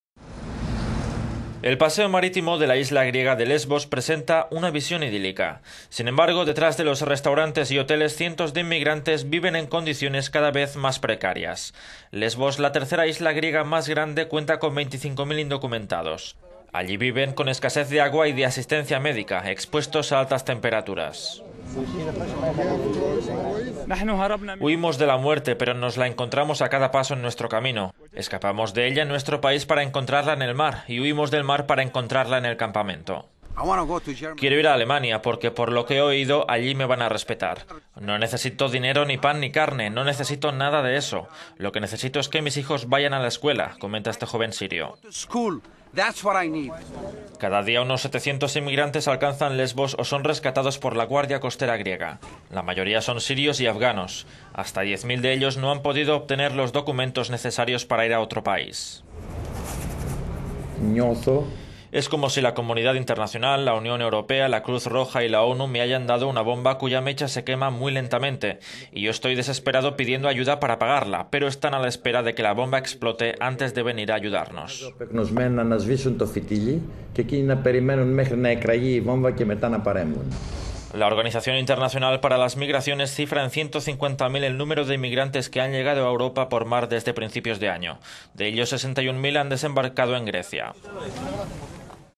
La inmigración sigue atizando a Grecia: en Lesbos viven ya 25.000 indocumentados
El paseo marítimo de la isla griega de Lesbos presenta una visión idílica. Sin embargo, detrás de los restaurantes y hoteles, cientos de inmigrantes viven en condiciones cada vez más precarias.
Lesbos, la tercera isla griega más grande, cuenta con 25.000 indocumentados. Allí viven con escasez de agua y de asistencia médica, expuestos a altas temperaturas

"Huímos de la muerte pero nos la encontramos a cada paso en nuestro camino. Escapamos de ella en nuestro país para encontrarla en el mar, y hu…